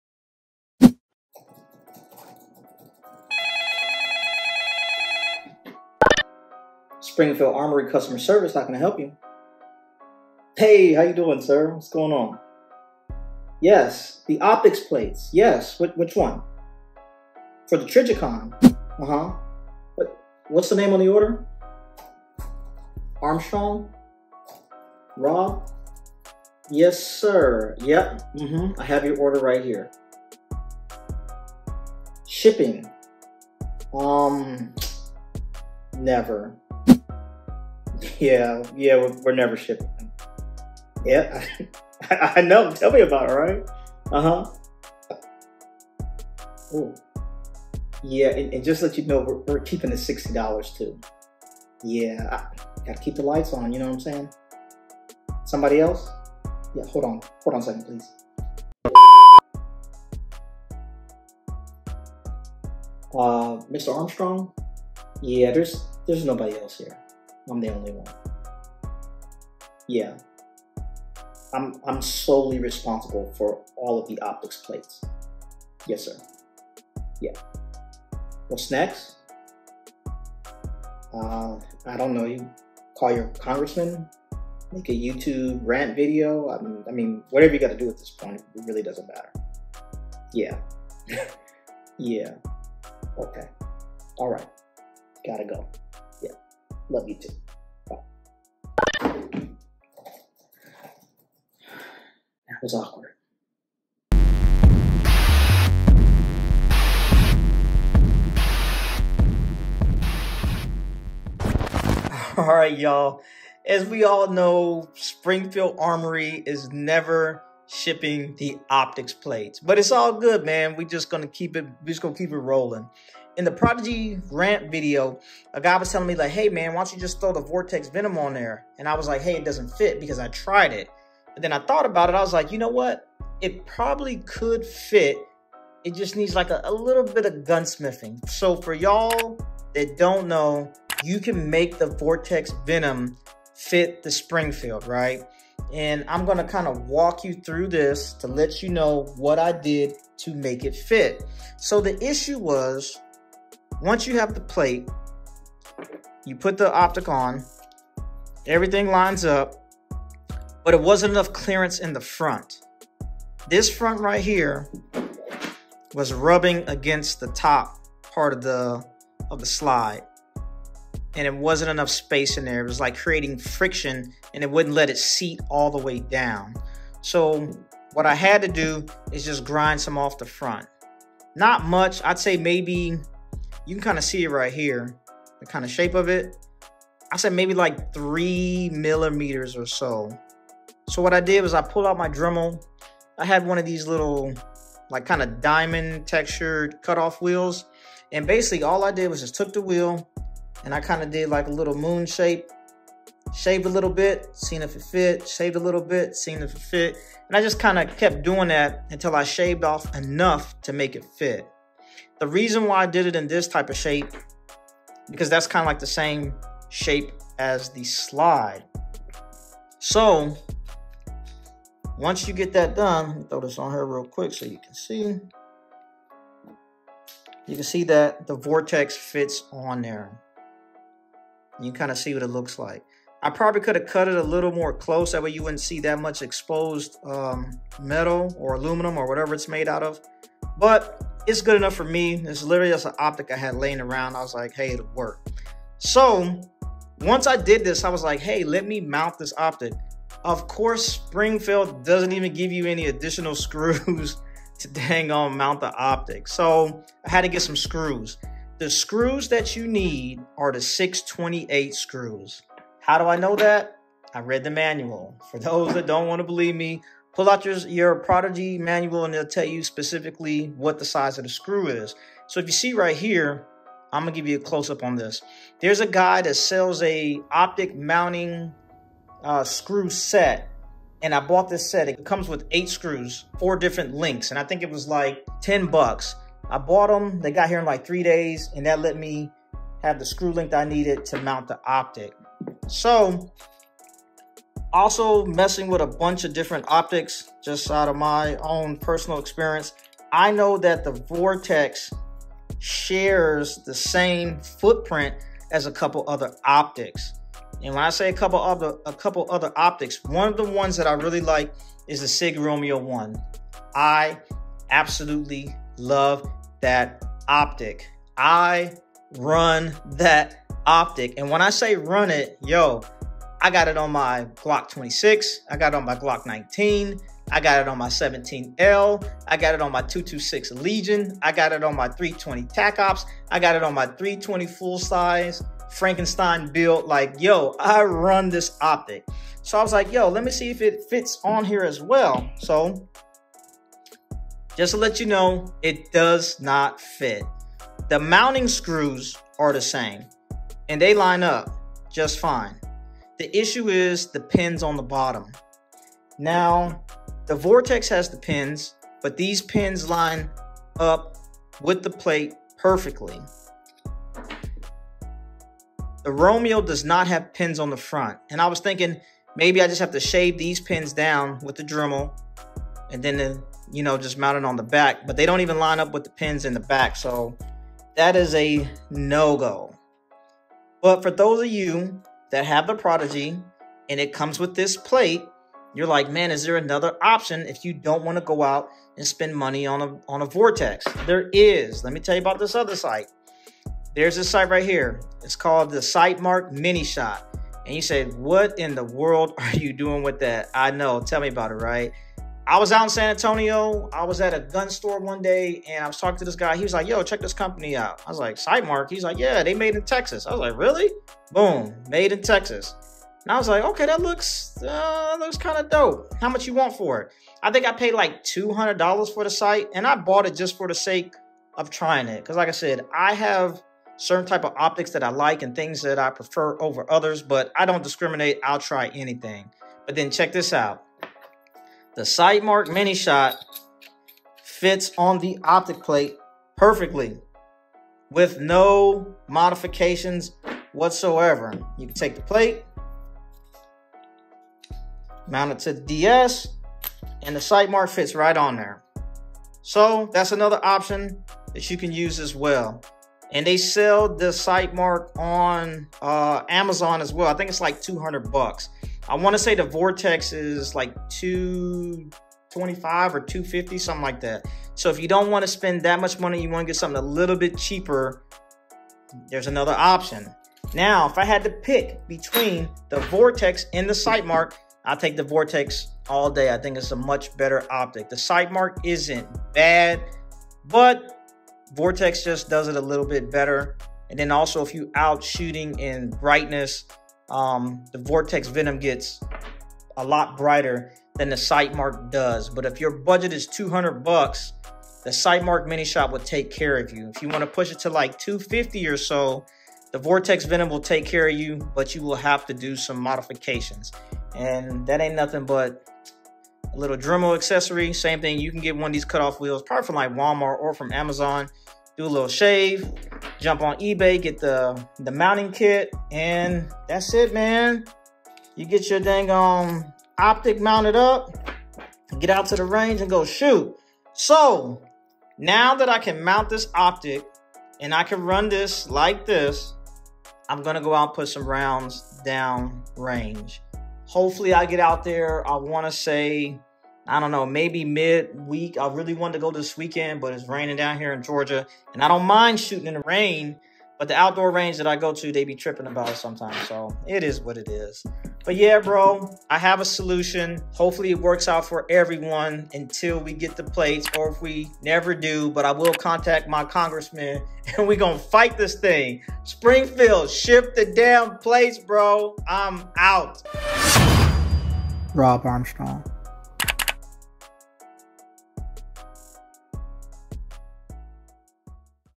Springfield Prodigy 1911 DS Optic Plate Solution | RobbArmstrong
As we all know, Springfield will *NEVER ship out the Optics Plates but... I found a way to make the existing plate work.